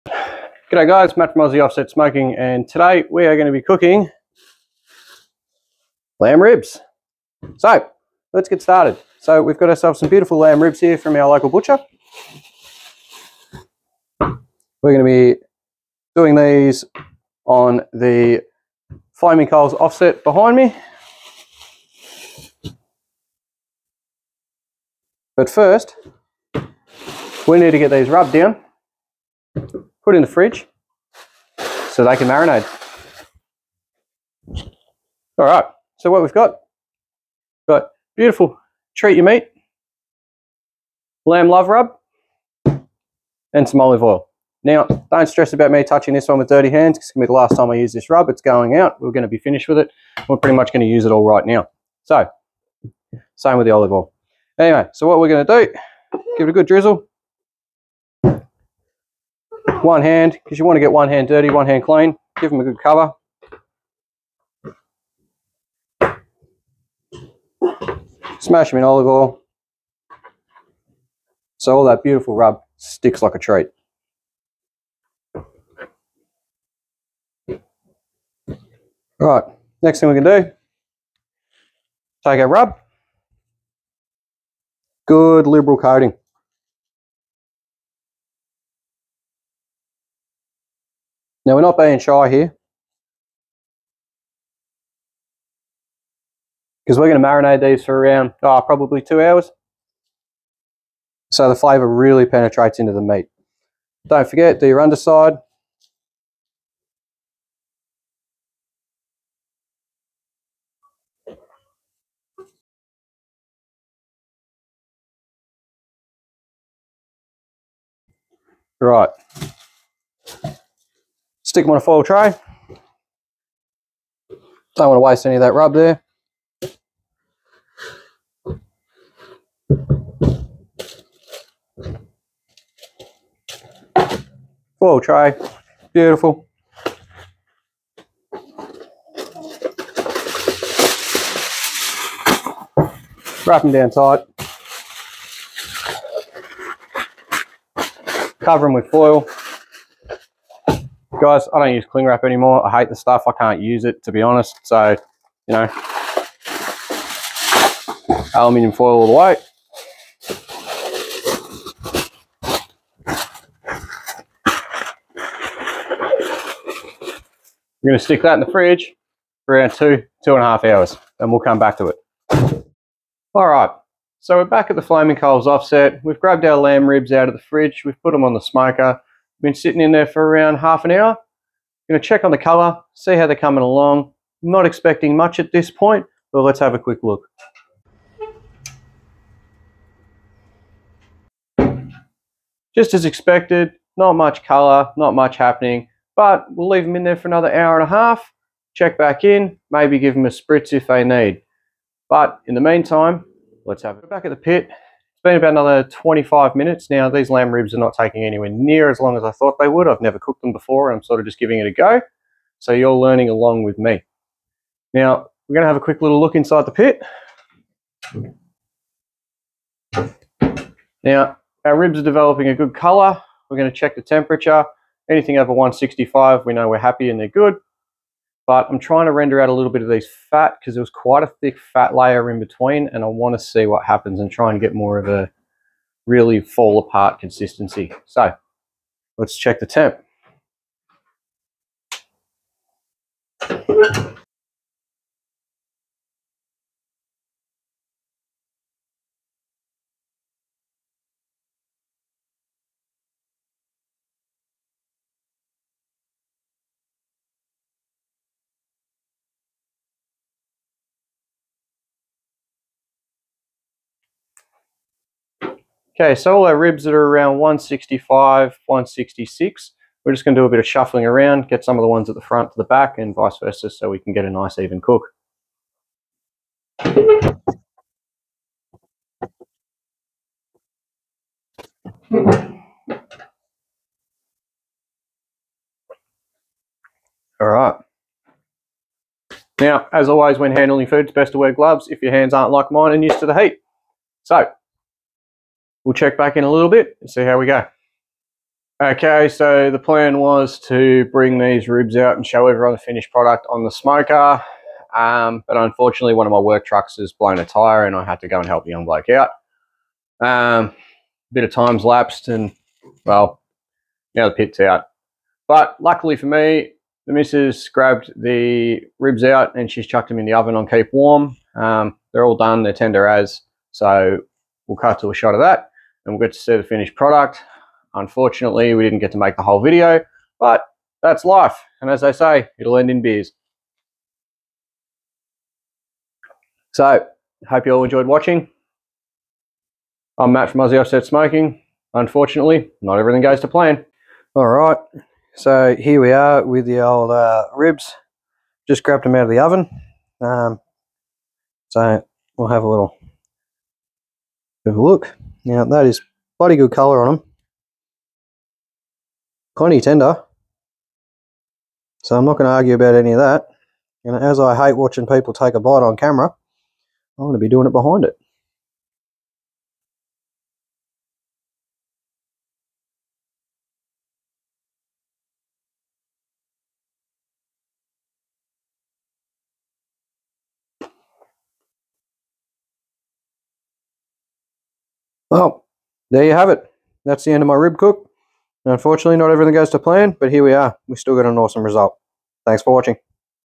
Lamb ribs on the offset
Lamb ribs cooked on our @flamingcoalsau offset smoker absolutely delicious